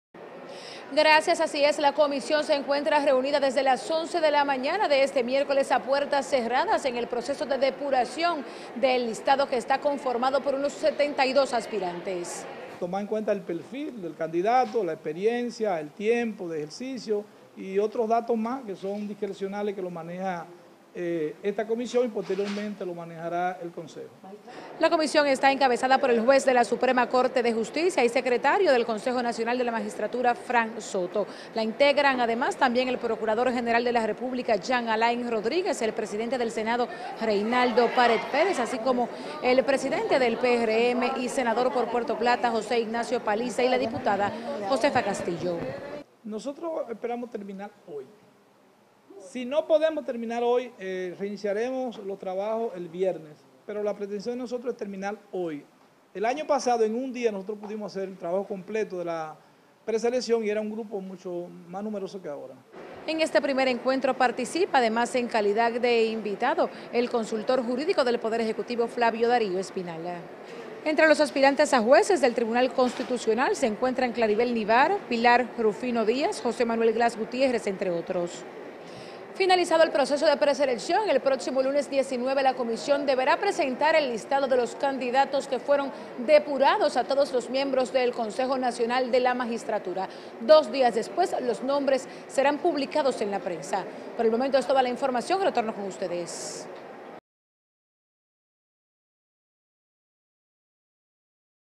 Inicia primer encuentro de la comisión para preselección candidatos TC - Noticias SIN
Este miércoles inició el  primer encuentro de la comisión designada para la preselección de los candidatos a sustituir los cuatro puestos vacantes del Tribunal Constitucional.

Esta comisión se reúne en la sede de la Suprema Corte de Justicia para iniciar los trabajos encomendados en el tercer encuentro del órgano que encabeza el primer mandatario.

La comisión está compuesta por los consejeros Reinaldo Pared Pérez, Jean Alain Rodríguez, José Paliza y Josefa Castillo bajo la coordinación del magistrado Frank Soto Sánchez.

Luego de evaluar a los aspirantes, el consejo se reunirá el 19 de este mes para aprobar la lista definitiva de quienes serán entrevistados y examinados.